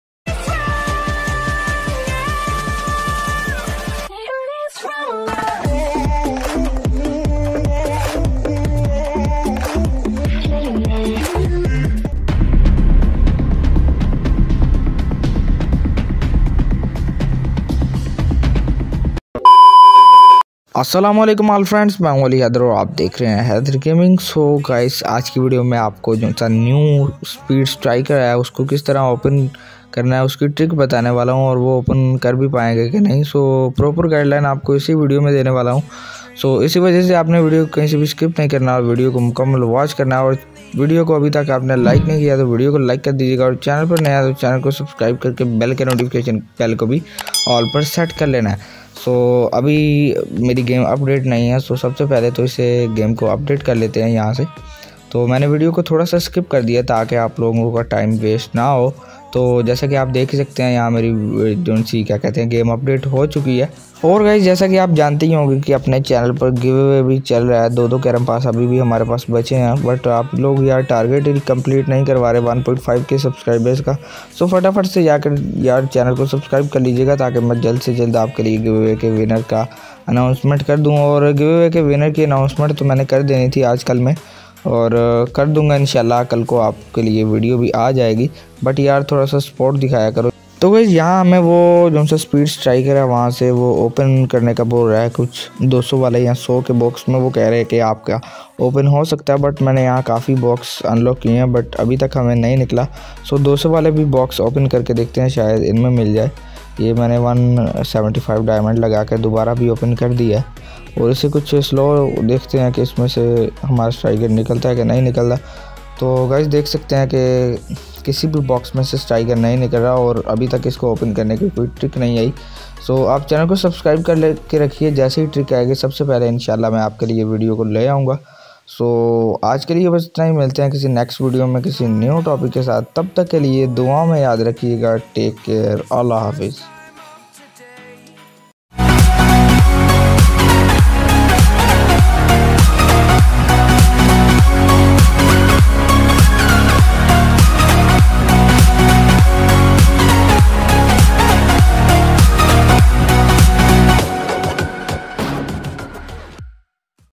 How to open speed striker 💯Free🔥 latest trick ||Haider Gaming786
So guys is video main mene apko New speed striker open karne ki trick btayi he so video ko enjoy kijie aur koi bhi part Samaj na aye to comment zarur kijie ga me reply zarur karu ga ❤🖤🔥

1:Like👍
3:Share😗

ali_haider78652

Music credit ✨
.NCS.